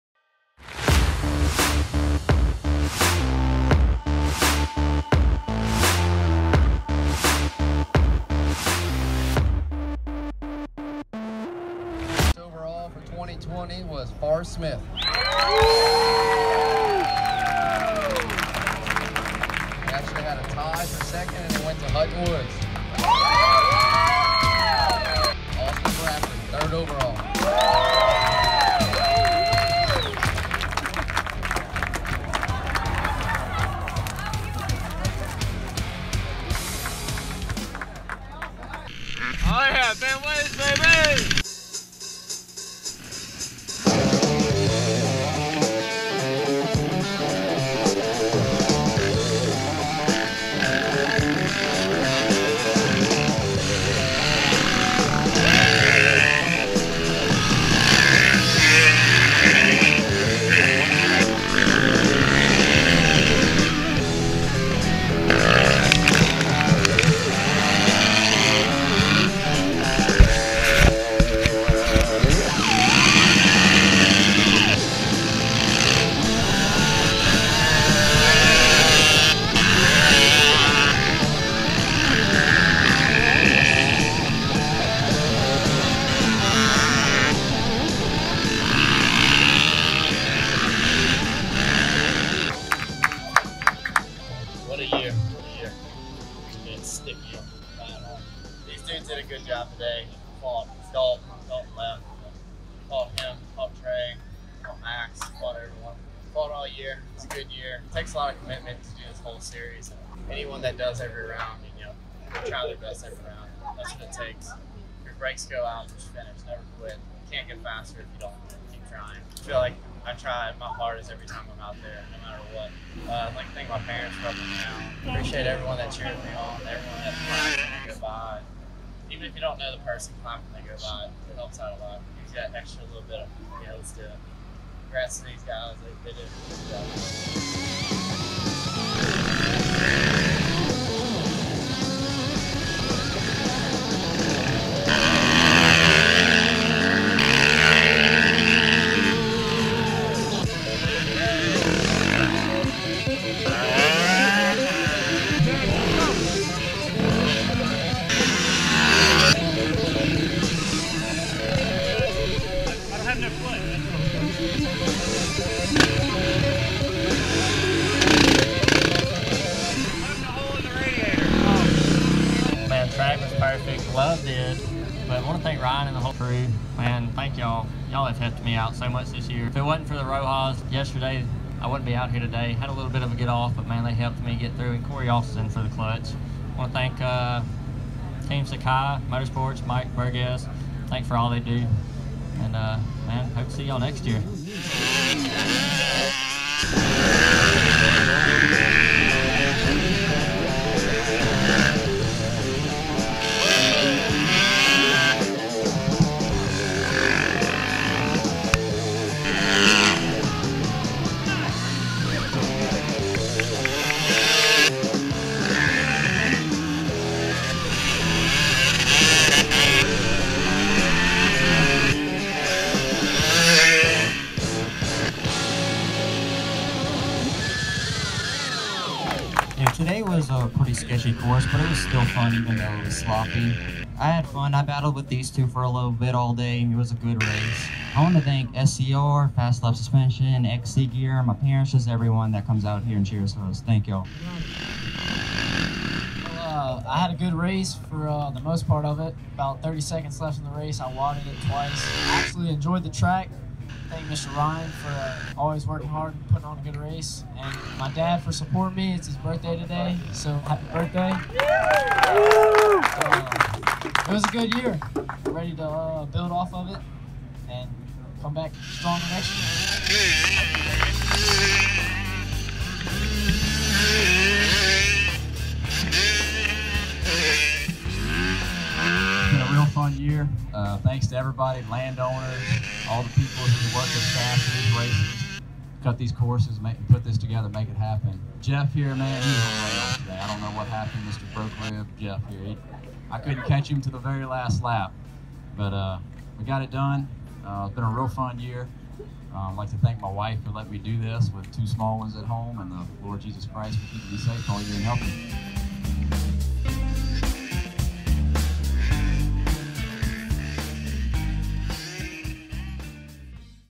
LACC Rnd 10 Van Way MP Podium Speeches
The tenth and final round of the 2020 LACC Championship series took place in Carencro, LA at Van Way Moto Playground.

Wet conditions made the course very slick in some areas.  Forrest Smith (Yam) took the event win and the overall year-end championship, his third time as LACC champion.  2nd overall for the year was Hutton Woods (KTM), and 3rd overall Austin Bradford (KTM).  Chance Mooney won the Open Expert class for the year and Andy Chance won the Senior A 30+ class.

Thumbnail photo of Forrest Smith by Jenny Comeaux.